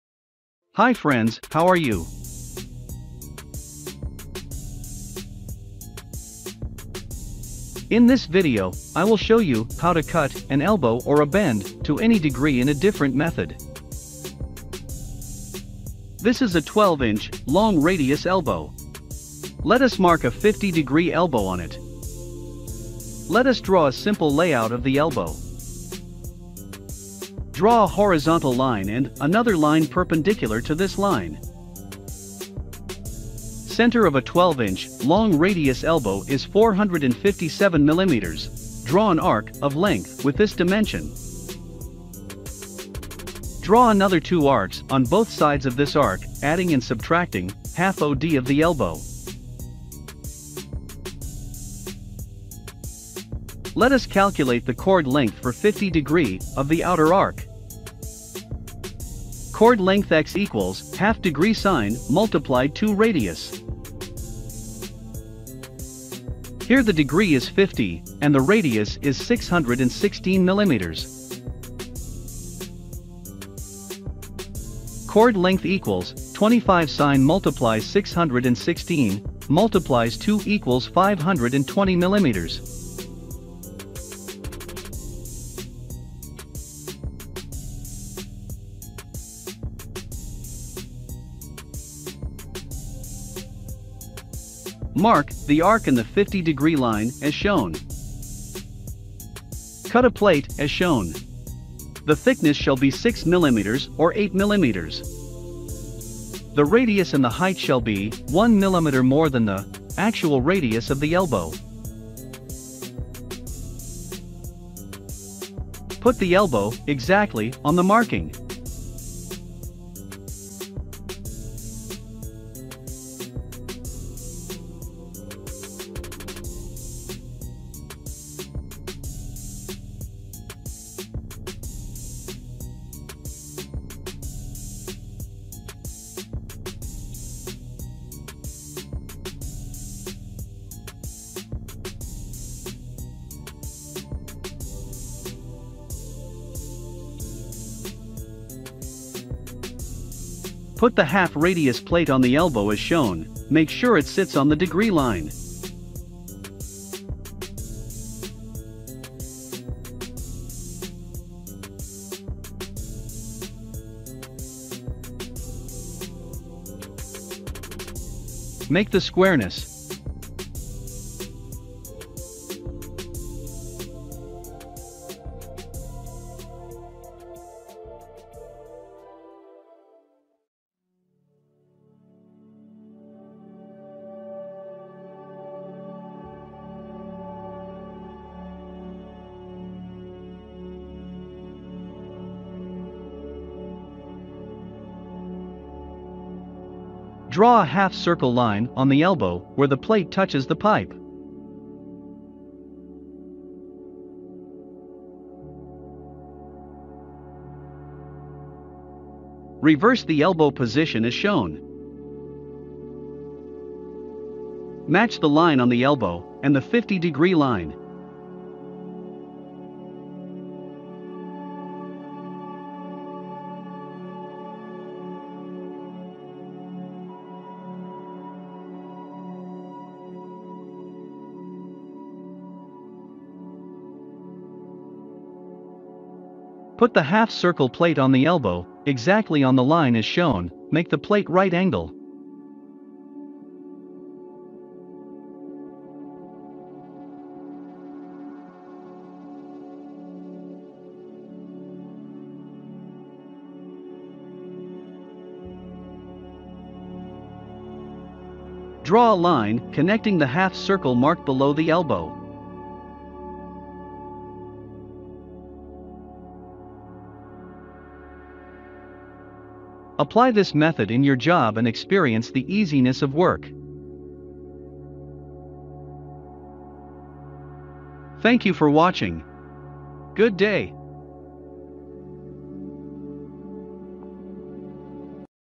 How to cut an Elbow to any degree Simple method
How to mark different degree elbows on an elbow or a bend, an easy, fast, and simple method for beginners.  @technicalstudies.